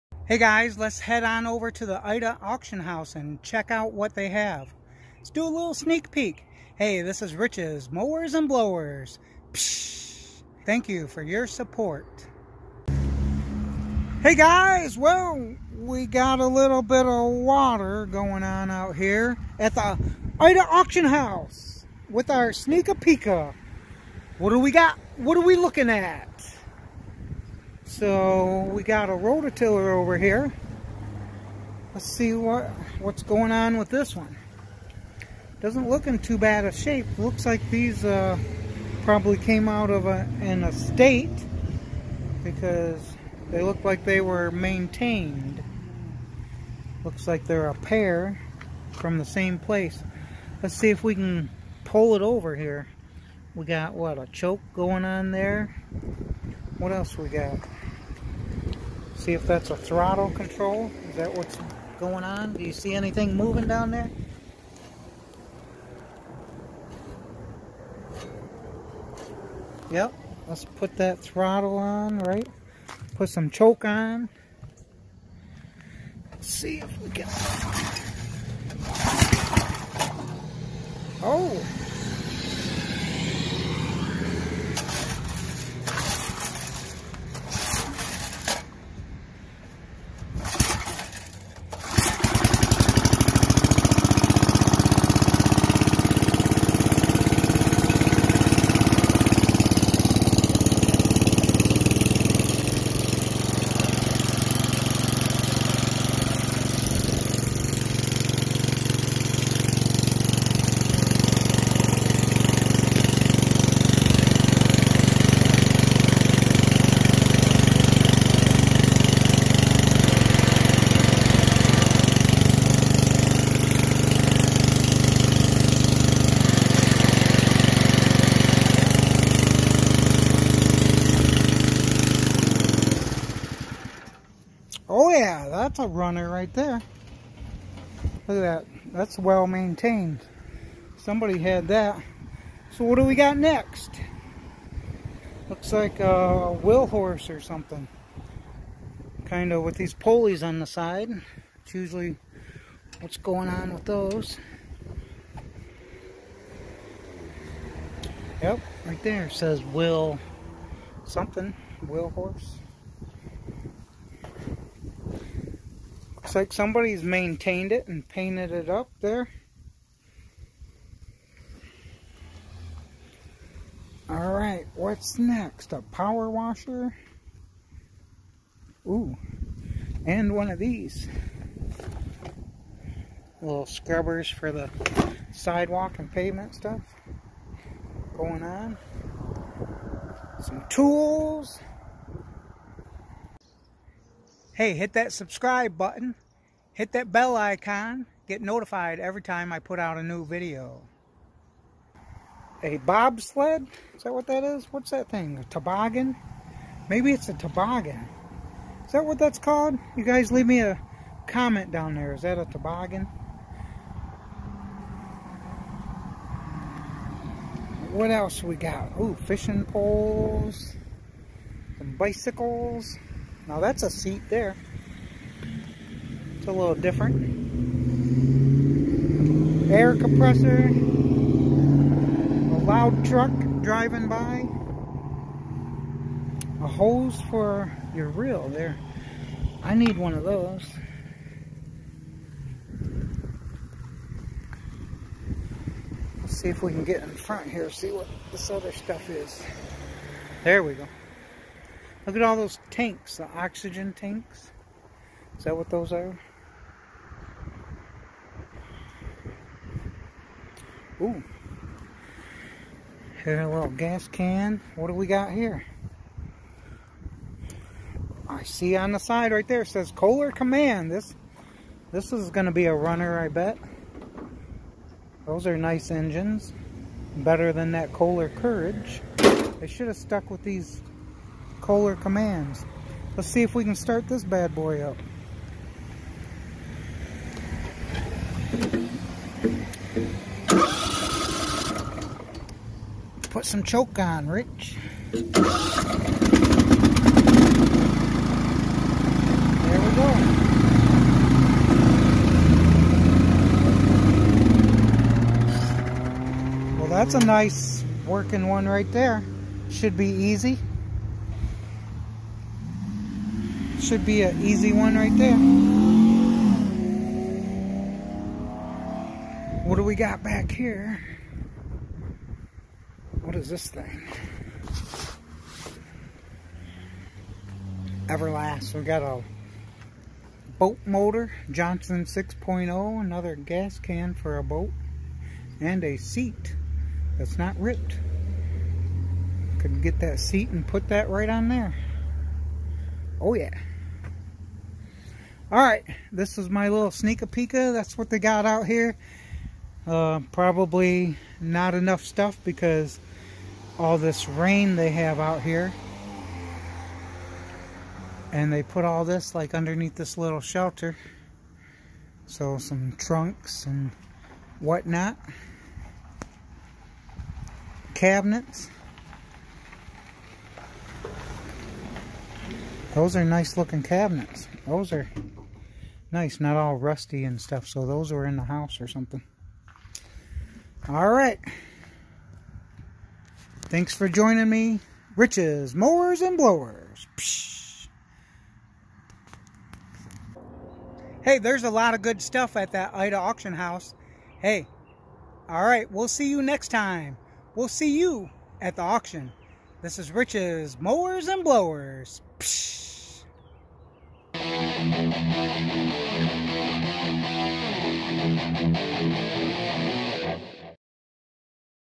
Exploring Hidden Gems Ida Auction House walk around with Rich's Mowers n Blowers Sneaka Peeka Pshhhh
Ida Auction House walking around with Rich's Mowers n Blowers  Testing and trying items up for bid. Will it Run? How much to bid?  Can it be sold for profit?